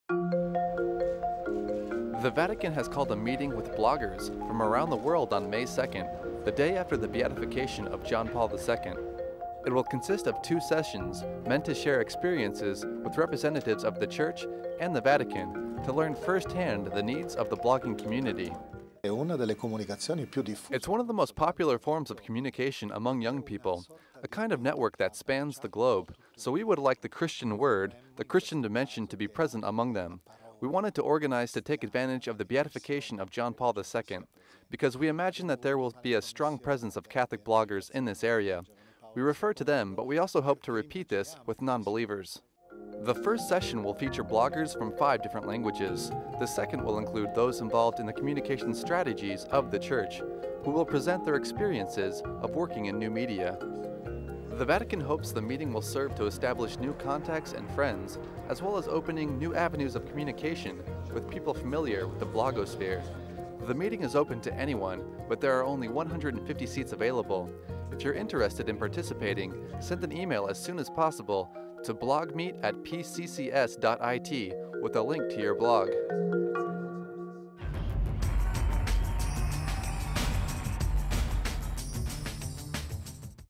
Are you a blogger? The Vatican would like to see your site!
The Vatican has called a meeting with bloggers from ar .